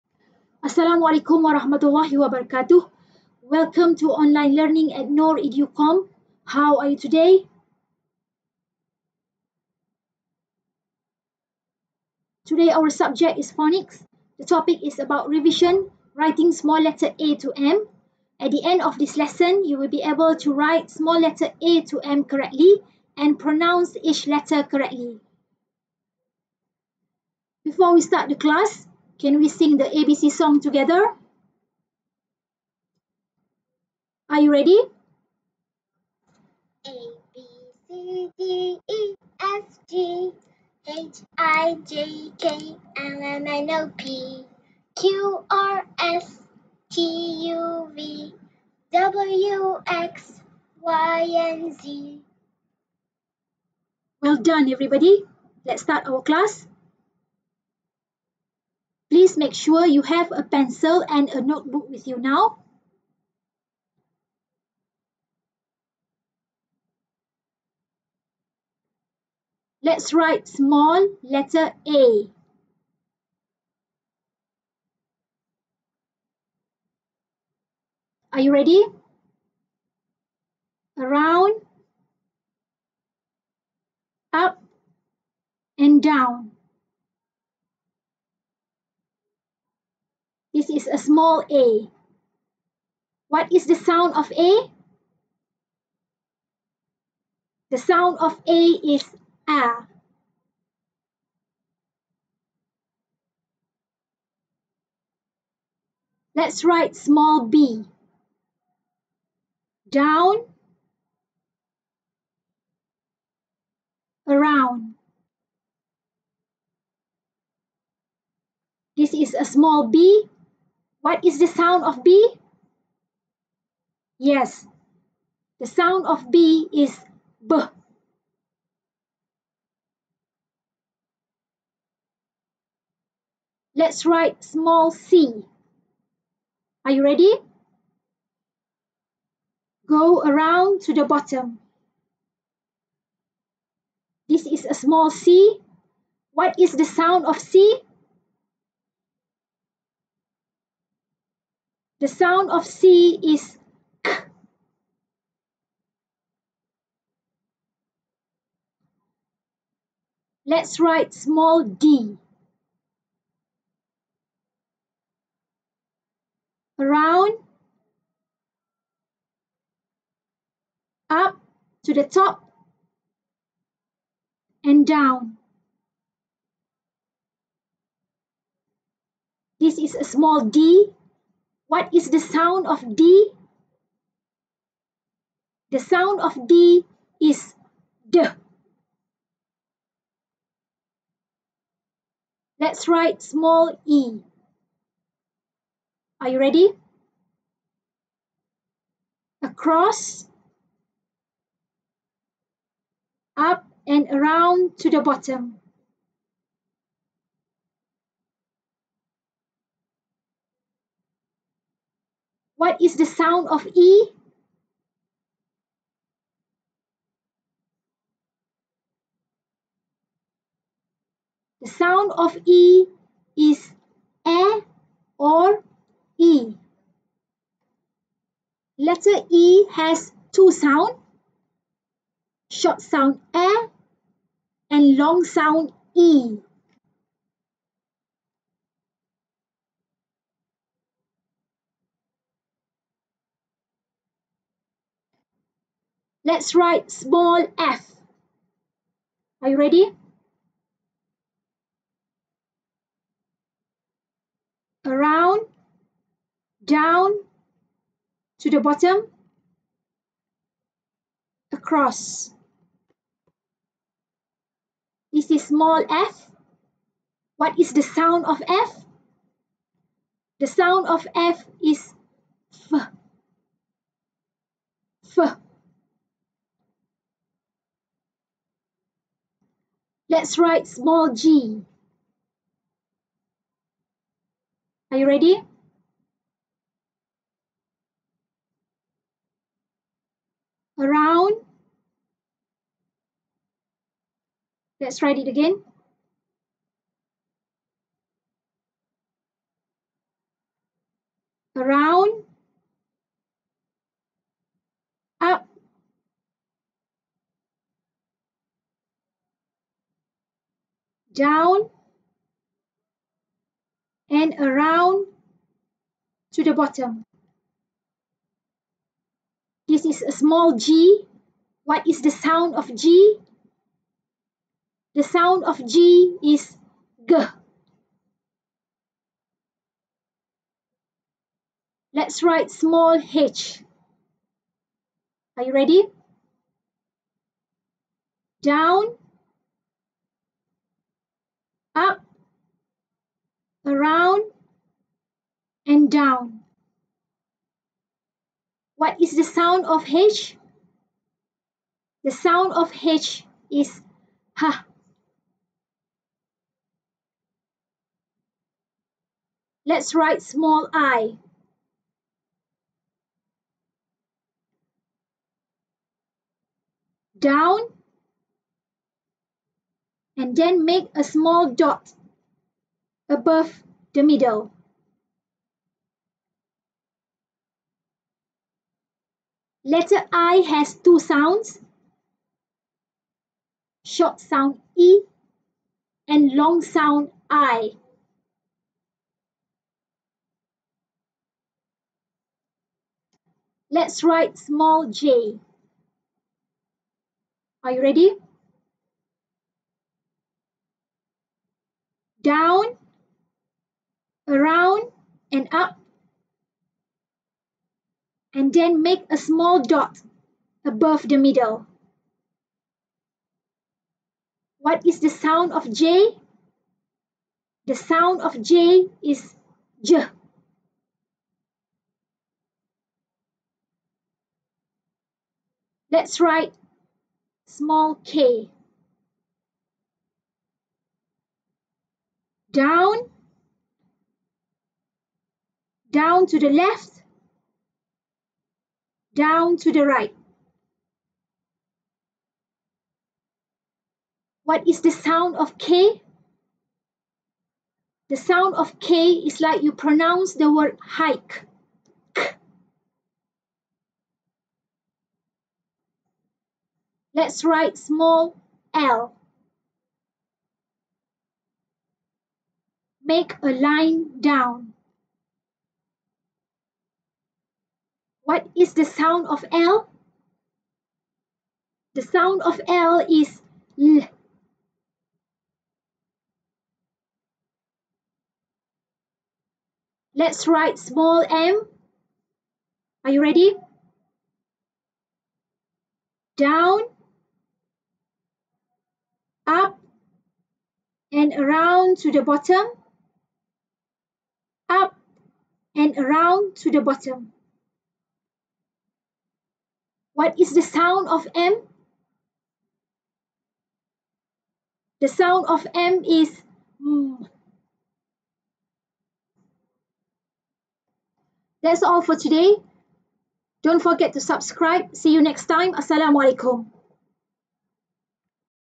KG1 Phonics Lesson#15 | Revision Writing Small Letter a to m | Online Learning at NorEduCom
Welcome to Online Learning at NorEduCom

Subject            : Phonics
Topic               : Revision - Writing small letter 'a' to 'm'
Objectives      : At the end of this lesson, you will be able to
- write small letter a to m correctly
- pronounce each letter correctly